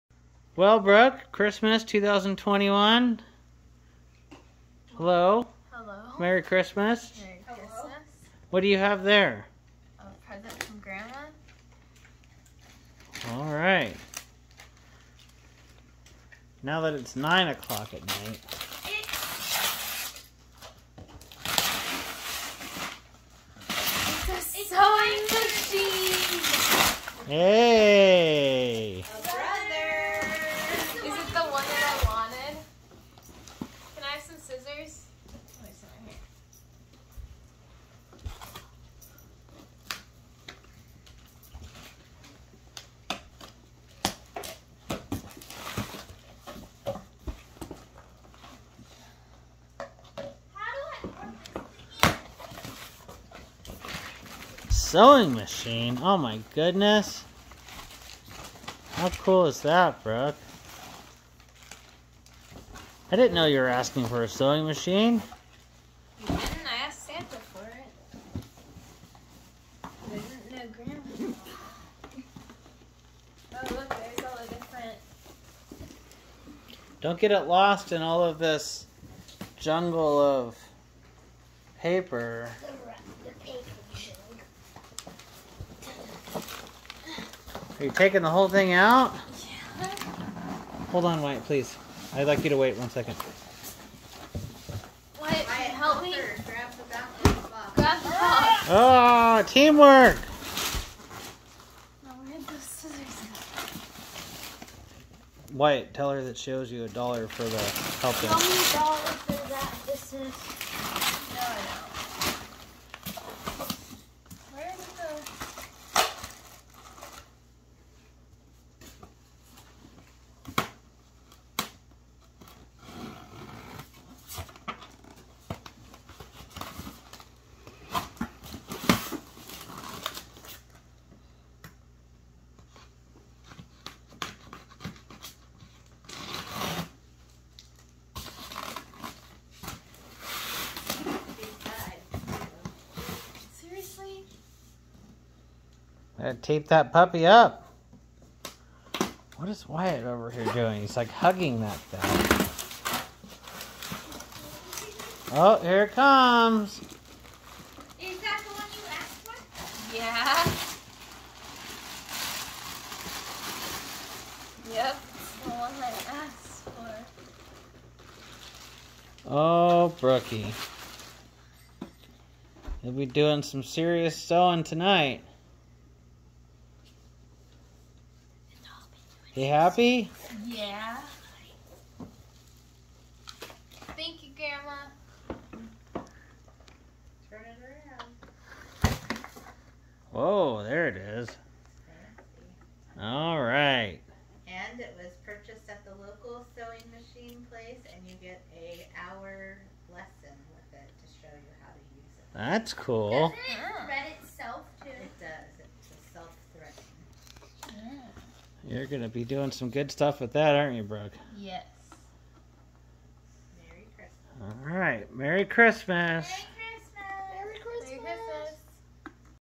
Brooke Sewing Machine Opening | Christmas 2021
Brooke opening her new sewing machine Christmas 2021 present. Merry Christmas and thanks Grandma Susan.